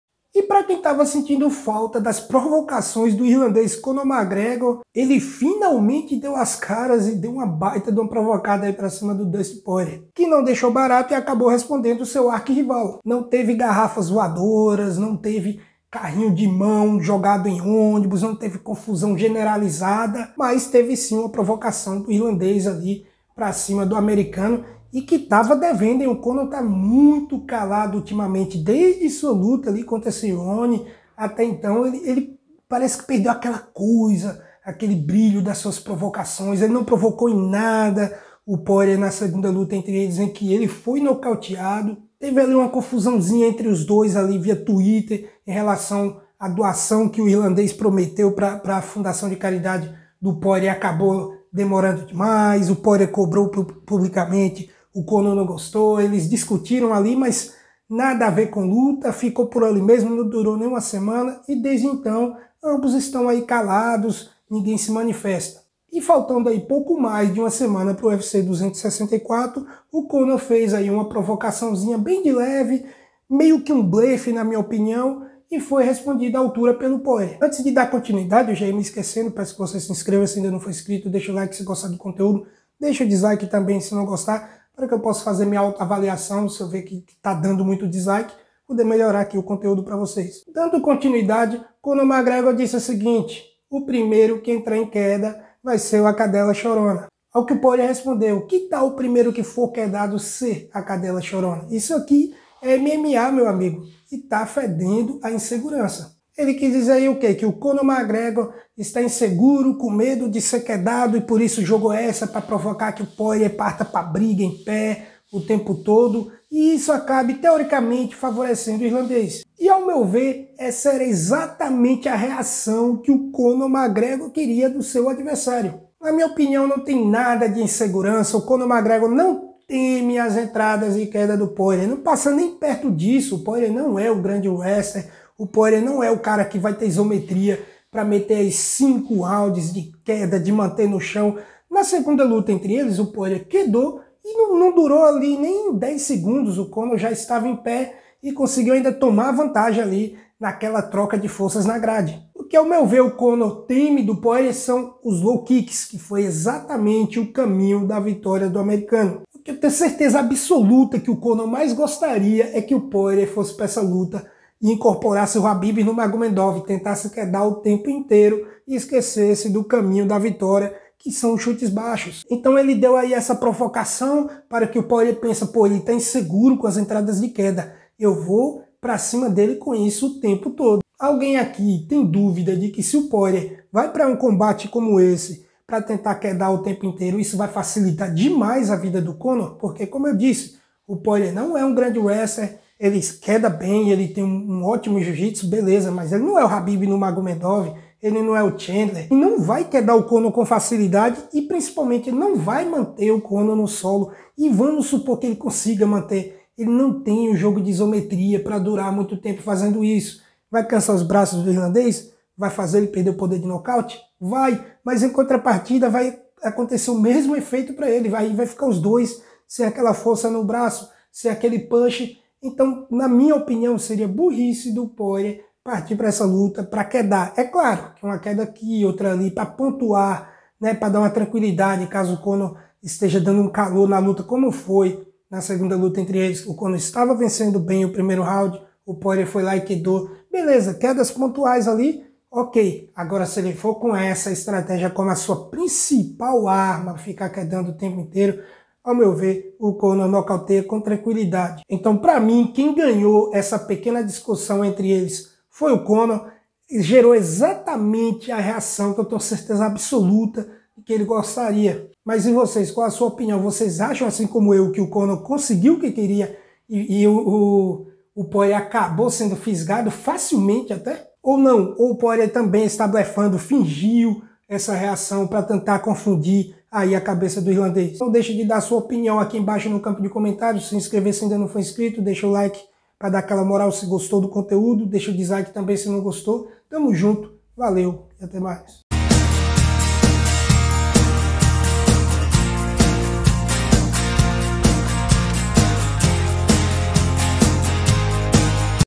TRETA ENTRE CONOR MCGREGOR E DUSTIN POIRIER
conor mcgregor anda muito sumido e pouco tem provocado seu proximo adversario, o americano dustin poirier onde fecharam a trilogia no ufc 264, mas agora o irlandês resolveu aparecer, provocou o diamante e recebeu resposta a altura e é sobre isso que vamos comentar no video de hoje no canal cotovelada giratoria.